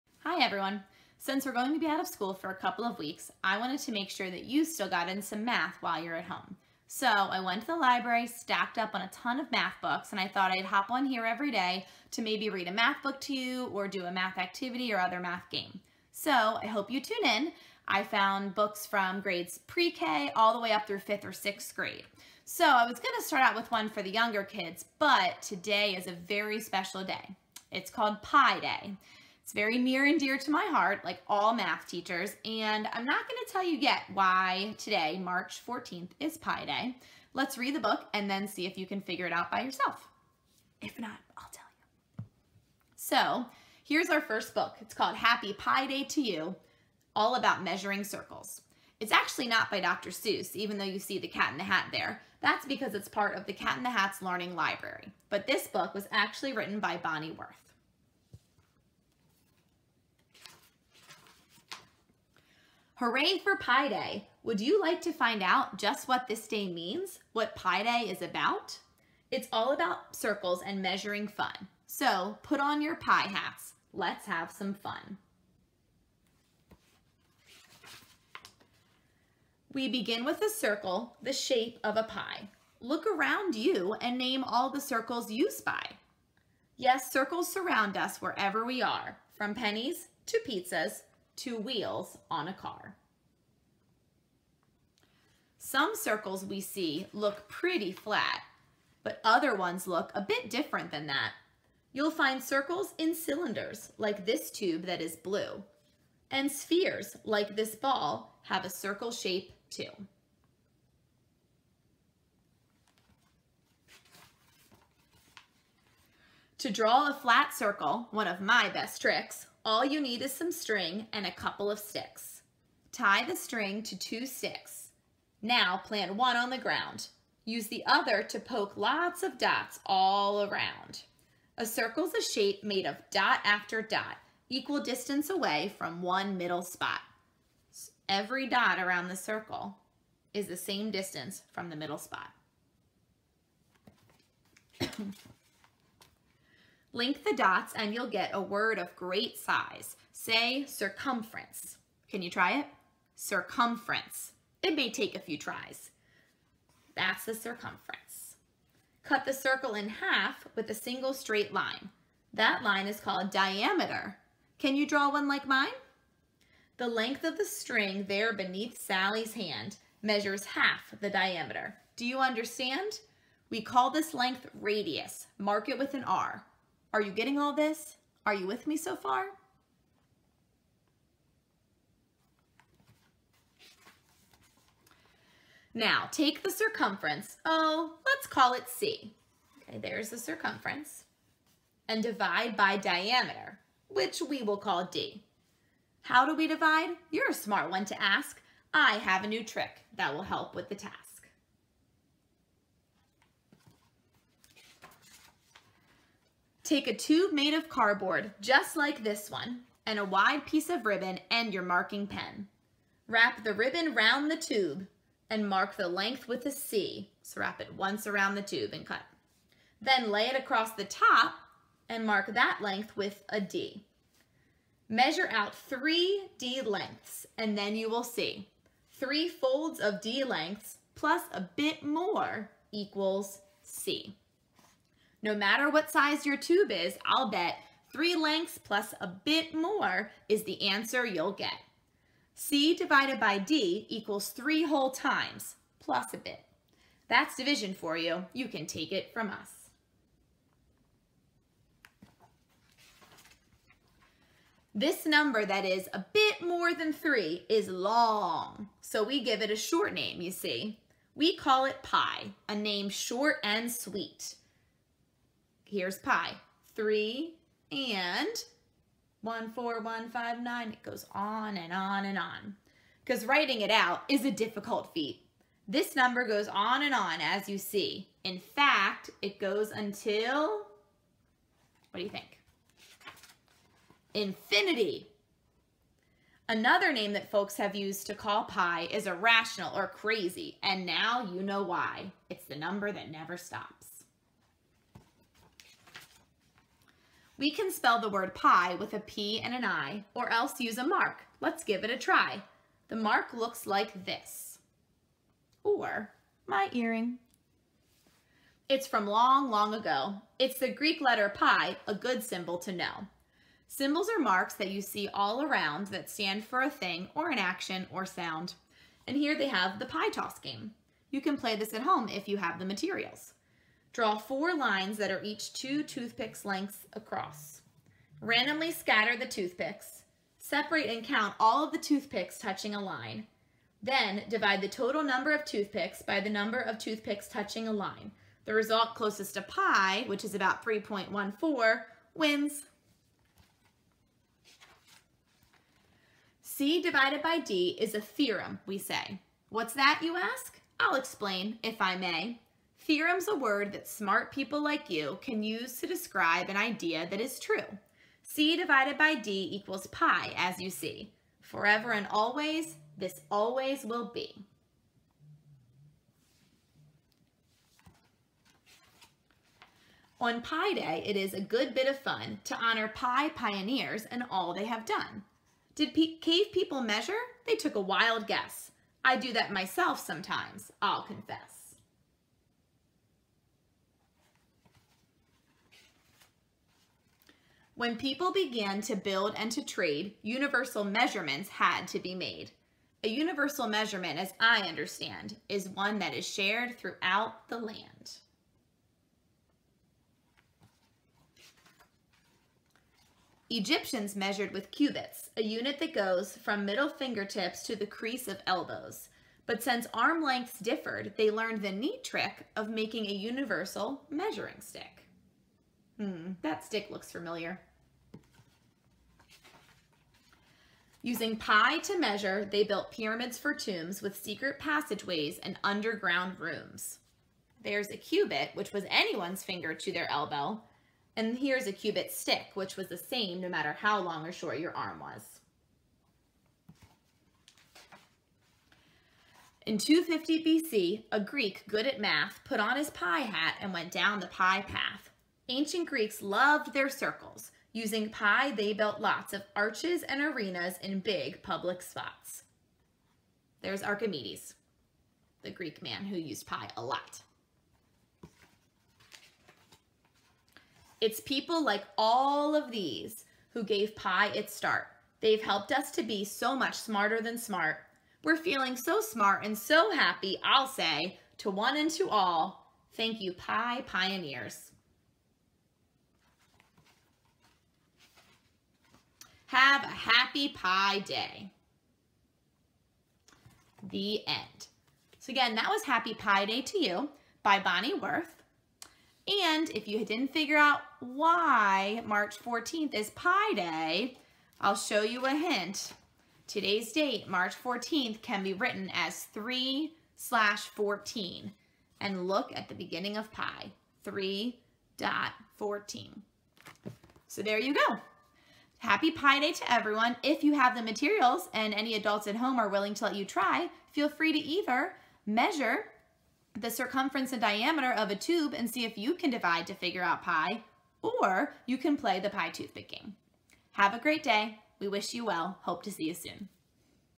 Miss Battersby Reads "Happy Pi Day to You!" - 3/14/20
Since we can't be in school right now, let's learn about math virtually! I hope to post each day while we are out of school to keep everyone thinking about math. Day 1: A reading of "Happy Pi Day to You!" by Bonnie Worth.